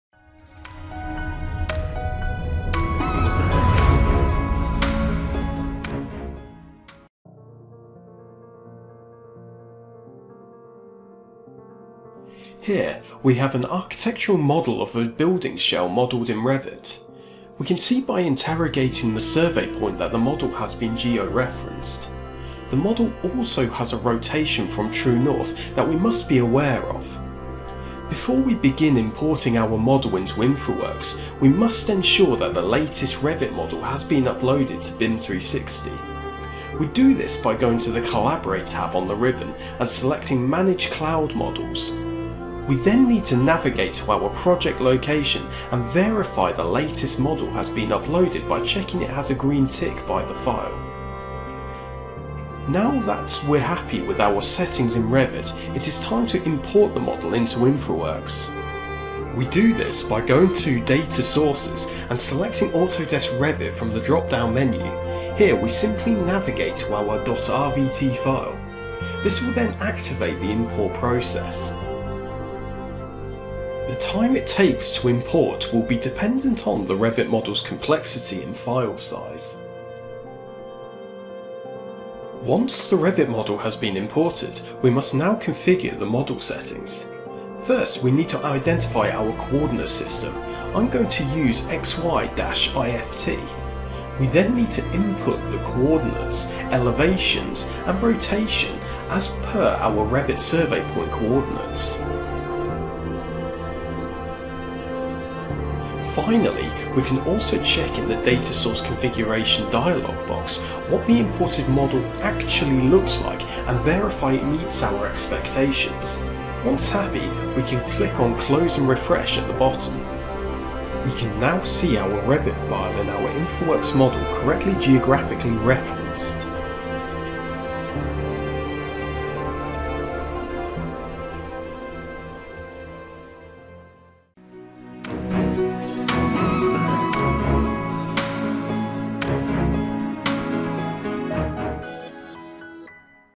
InfraWorks Importing A Geospatially Correct Revit Model
This video demonstrates the ability to import a Revit.rvt model file into Autodesk InfraWorks using the correct coordinates.